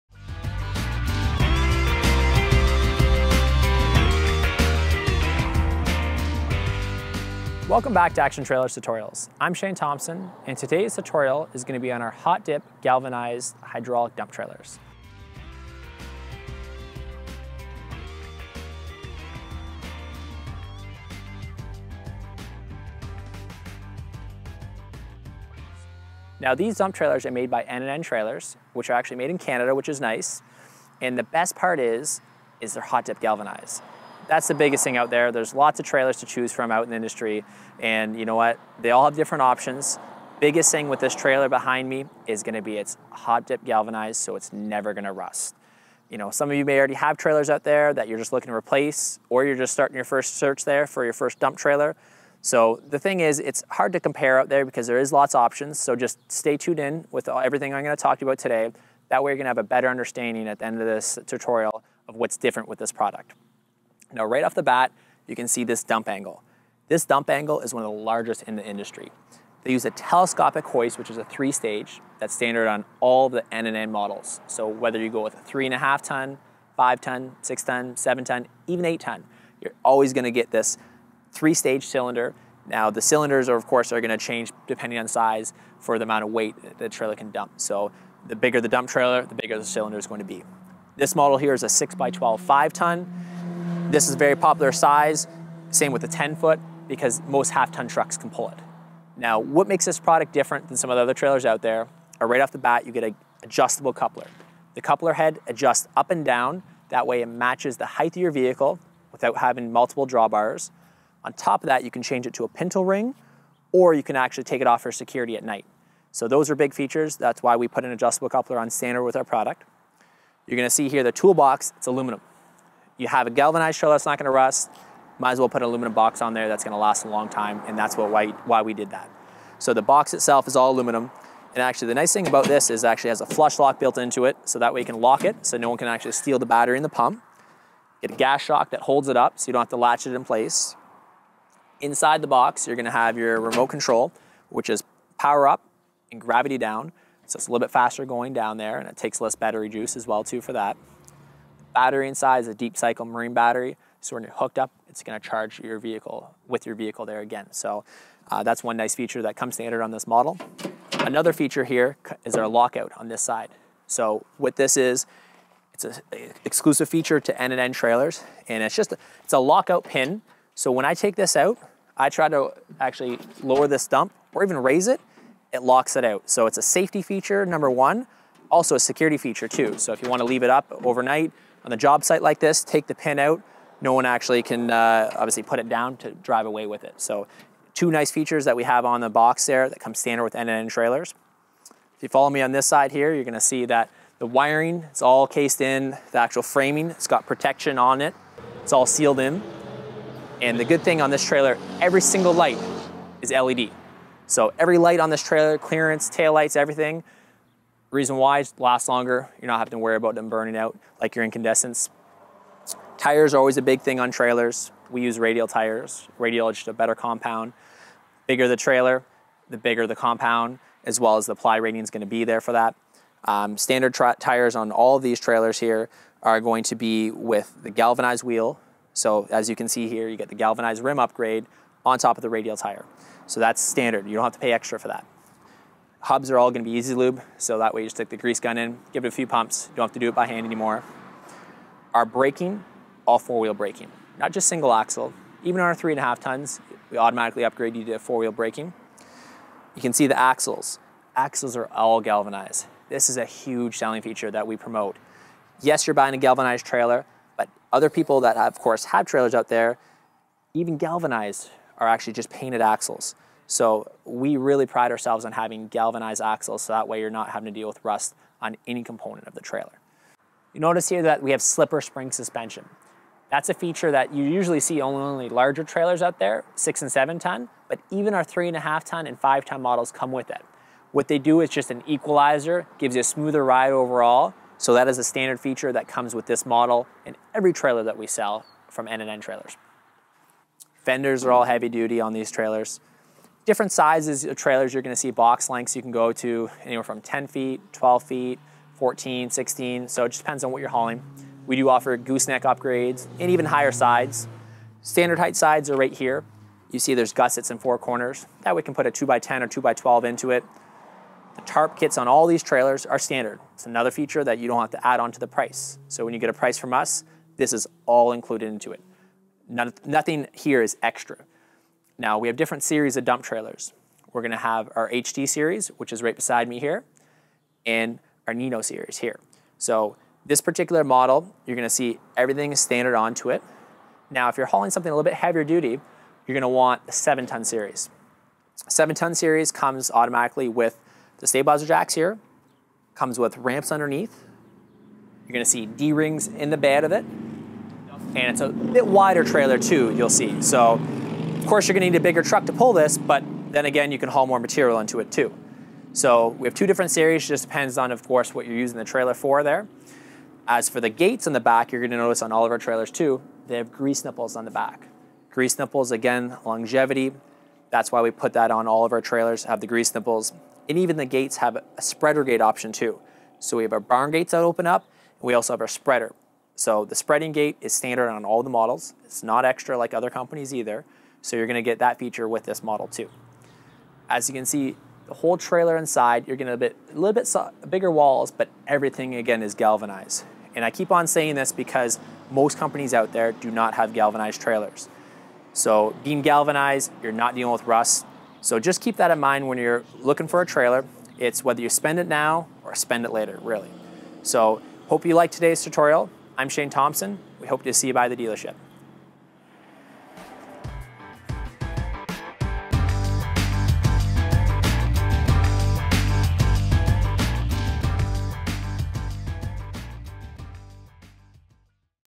Galvanized Hydraulic Dump Trailer - ACTION TRAILER SALES
NEW N&N hot dipped galvanized dump trailers come in all sizes from 3.5 ton, 5 ton, 6 ton, 7 ton, . These trailers come with more standard features than most competitors dump trailer models, and being fully galvanized it will NOT RUST !